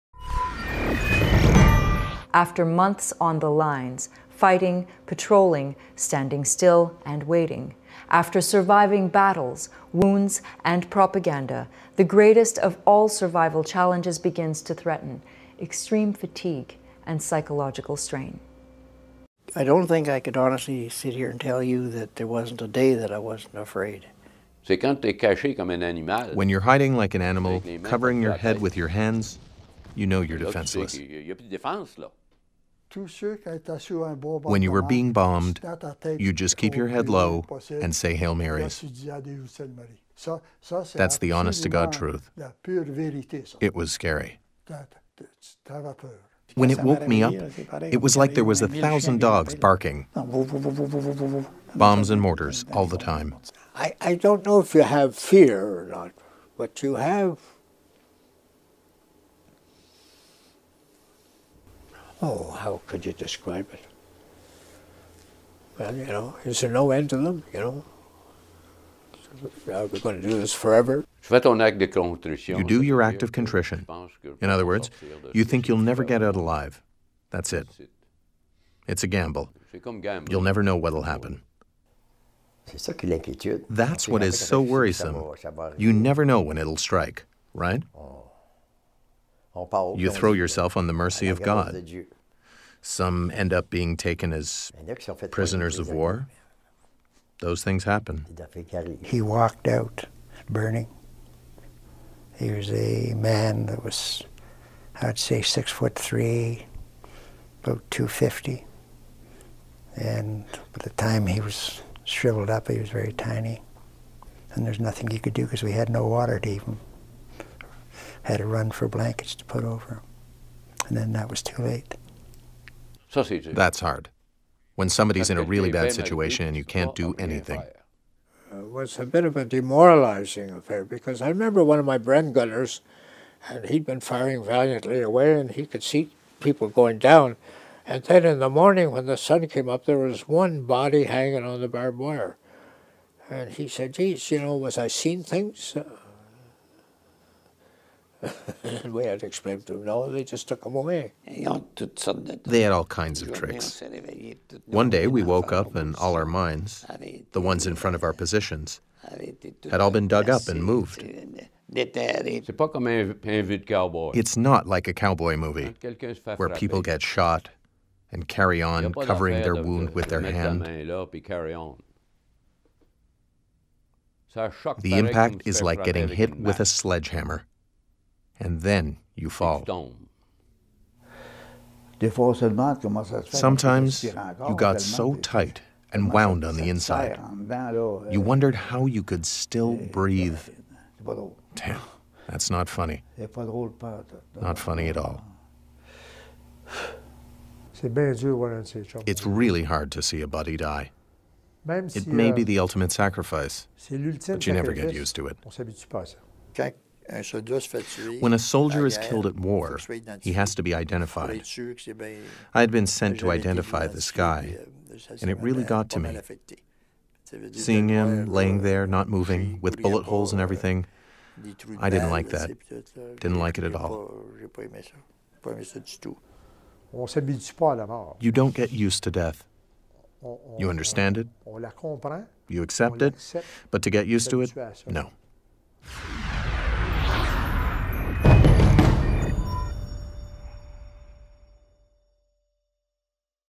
The Land of the Morning Calm - Part 3: Surviving - Psychological Strain
After months on the lines, fighting, patrolling, standing still, and waiting; after surviving battles, wounds and propaganda, the greatest of all survival challenges begins to threaten: extreme fatigue and psychological strain.